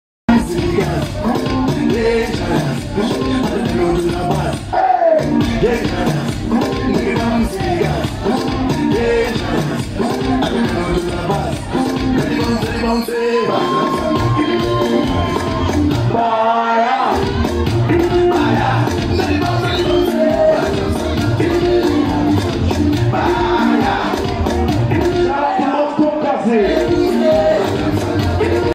kah dance kekutamba nemunhu asiri wako mubhawa
kutamba nemunhu asiri wako mubhawa. haubate wenge uchidyira sweet mubepa